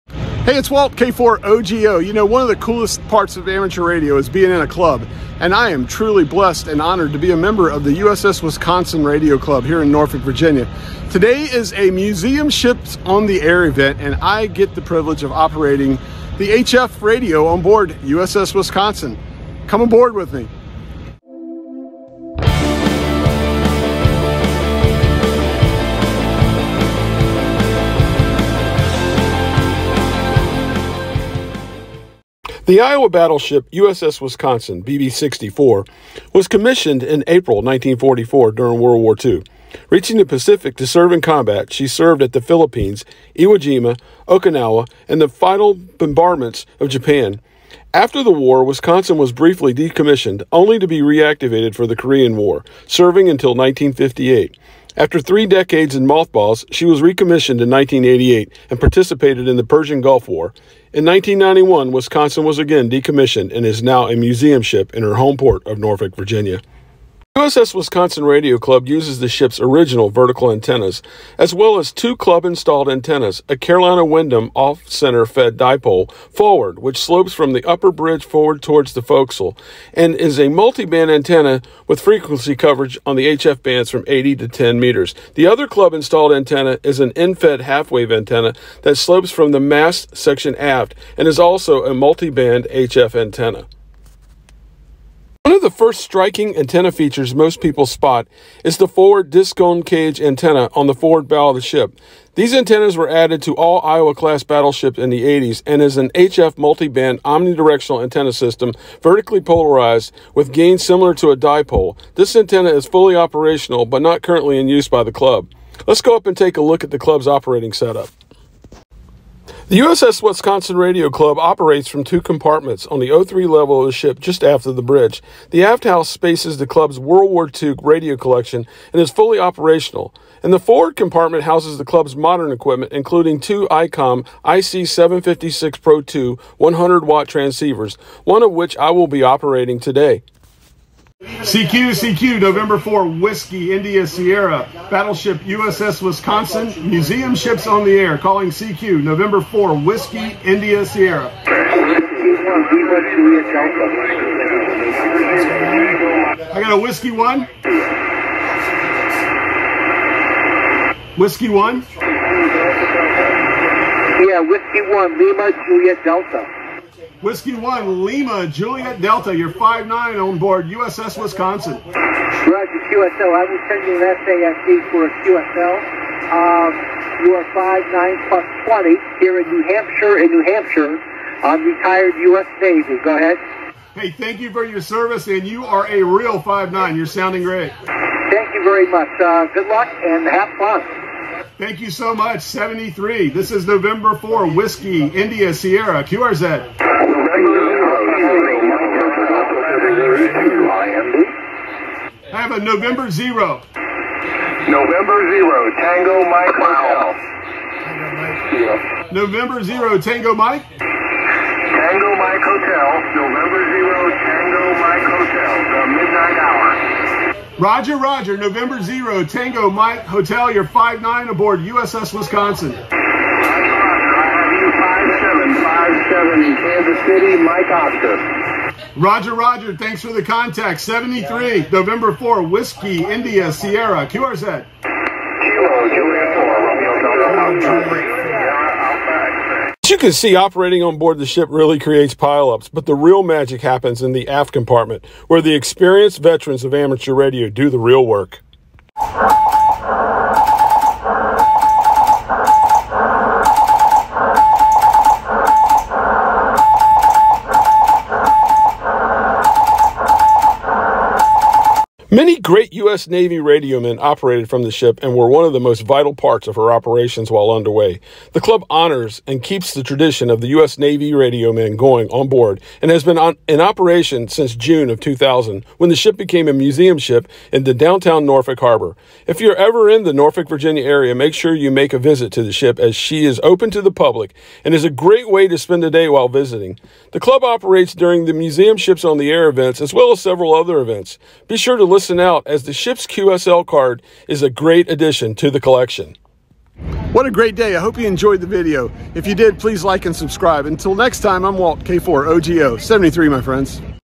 Ham Radio Onboard the USS Wisconsin
Operating the club station for the Museum Ships on the Air event. The USS Wisconsin Radio Club.